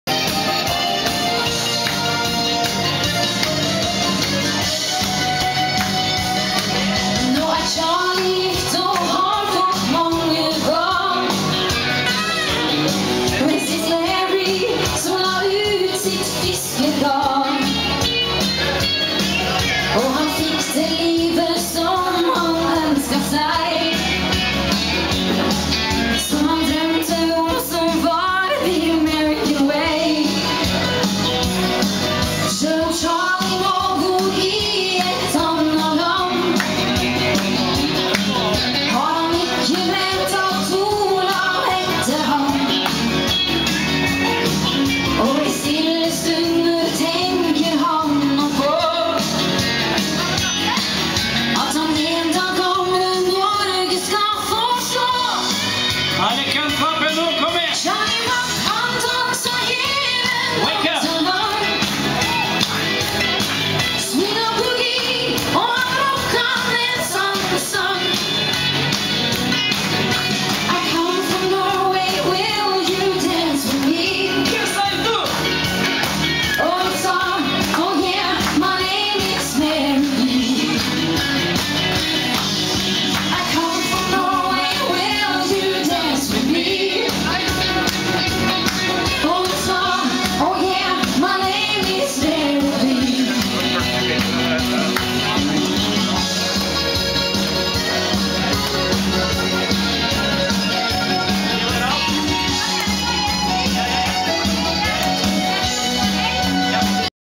Gro Anitta
Gro Anitta fra det Norske band Contrast synger på La Fiesta.